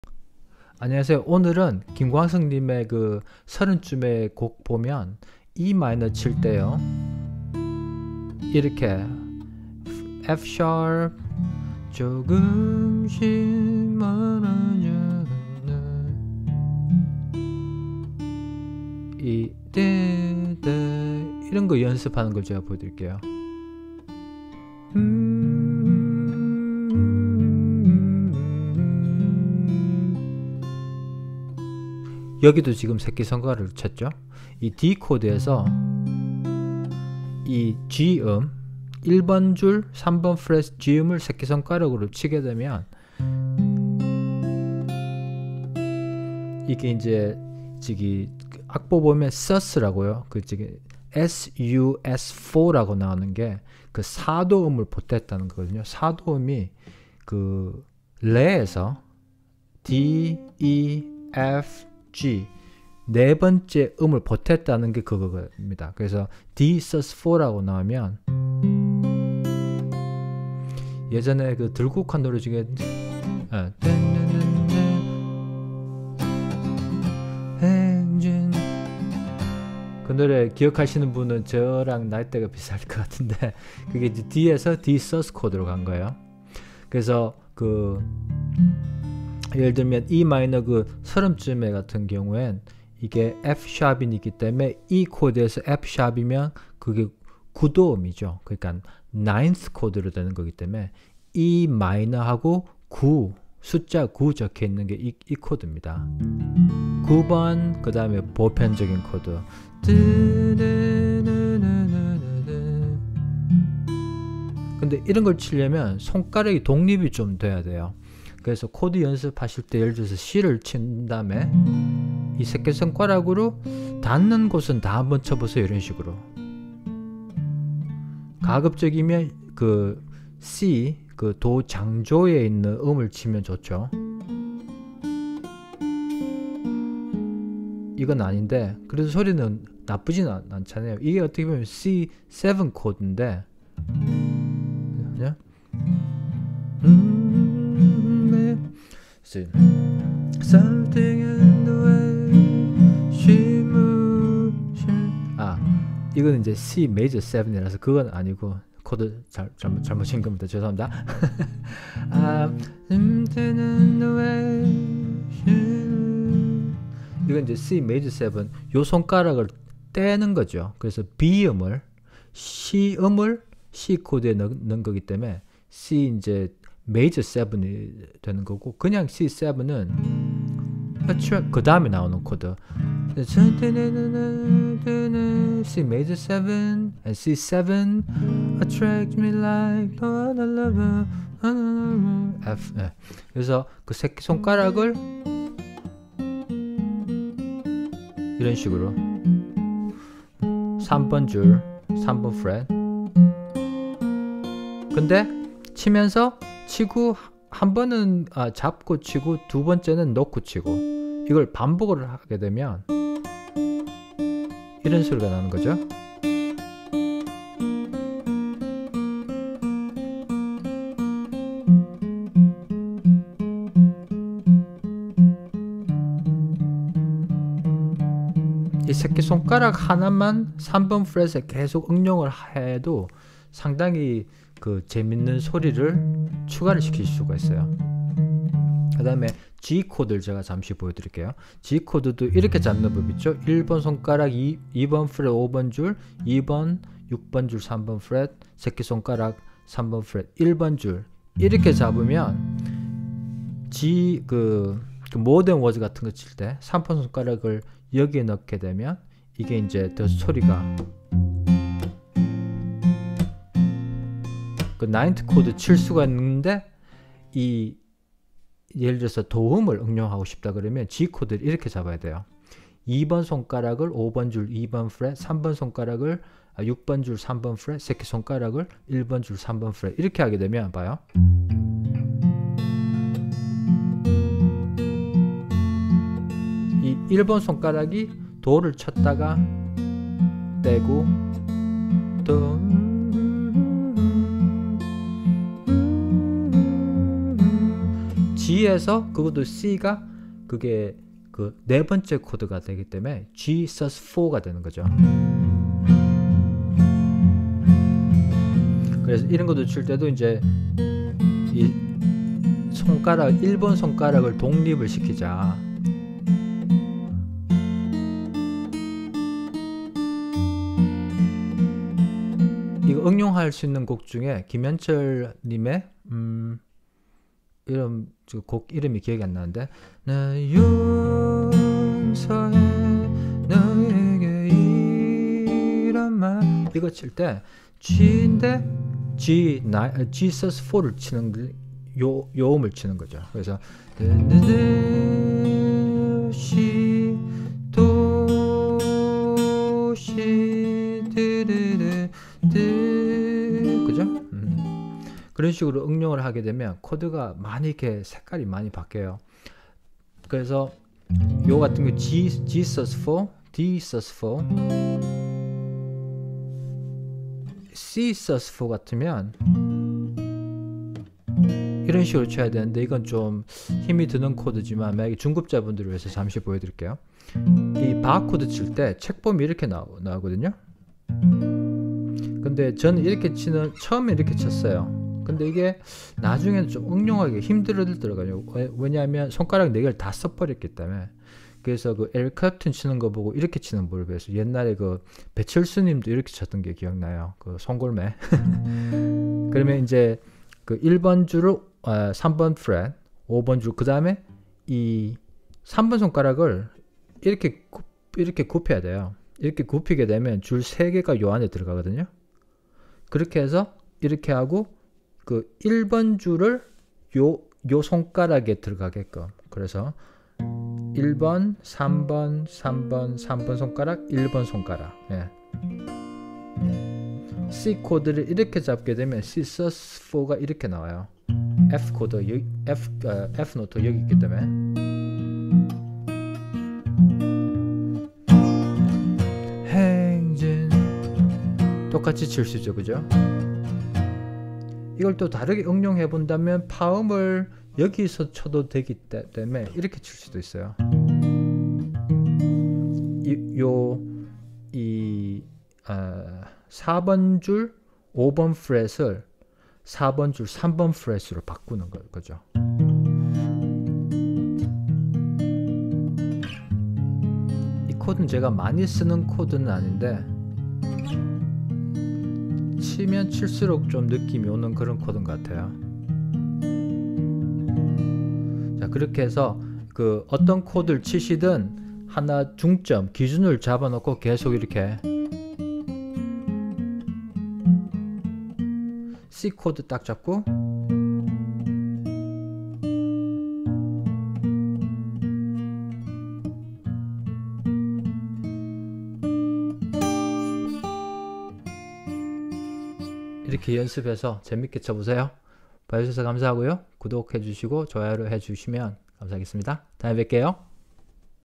맞나는 음 손가락 연습 - 9, sus4 코드
이 손가락 연습으로 Em9, Dsus4 같은 코드 쉽게 치세요. 기타치기가더 재미있어집니다.